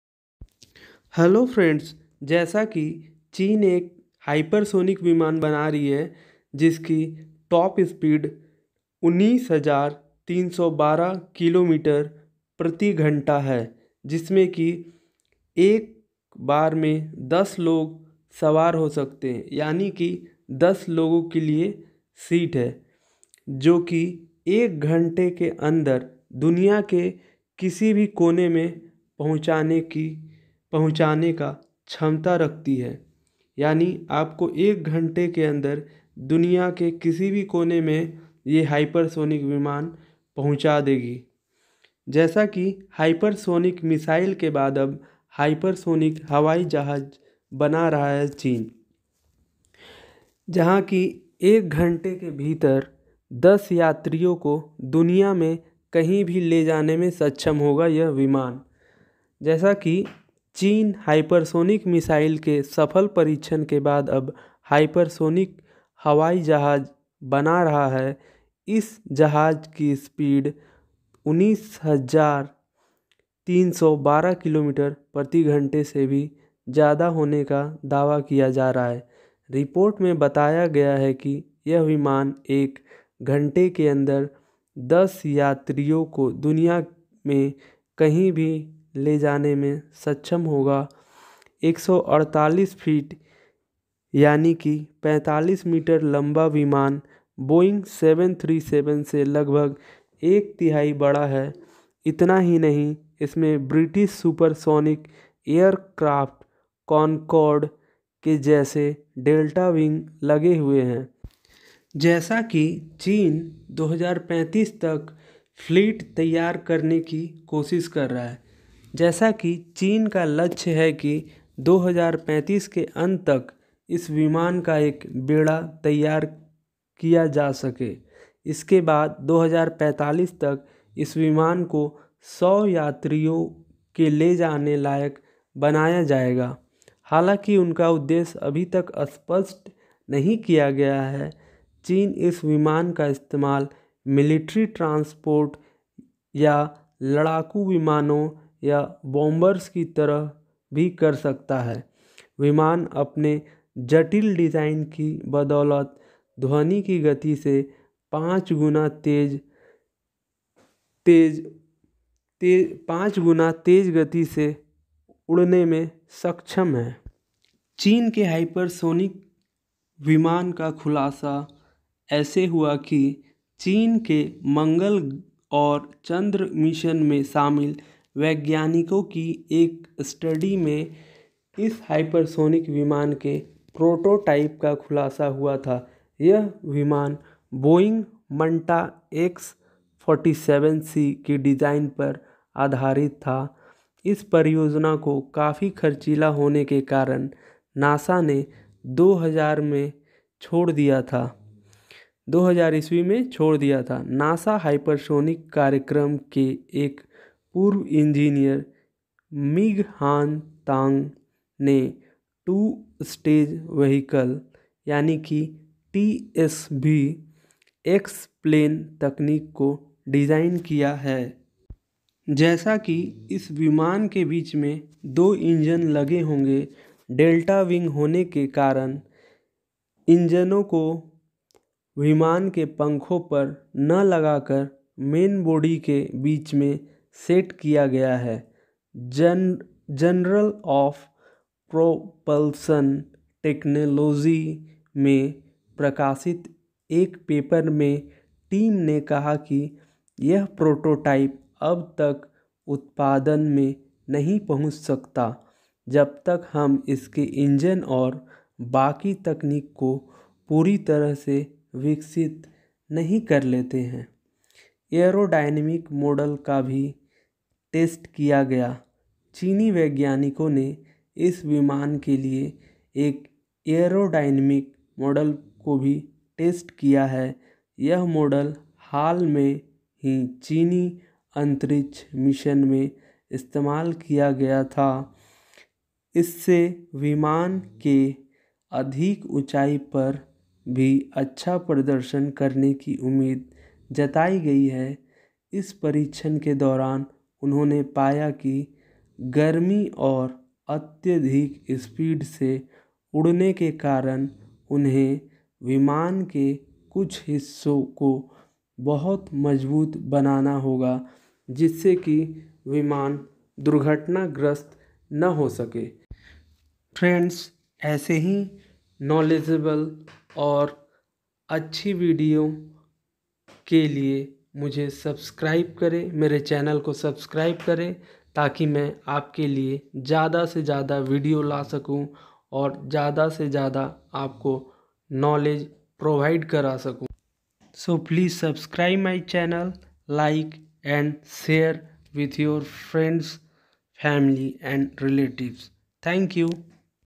CHINA UPCOMING HYPERSONIC AIRCRAFT PROMISES JUST 1 HOUR TRAVEL TIME TO ANY DESTINATIONS OF THE WORLD
CHINA'S UPCOMING HYPERSONIC AIRCRAFT PROMISES JUST HOUR TRAVEL TIME TO ANY DESTINATIONS OF THE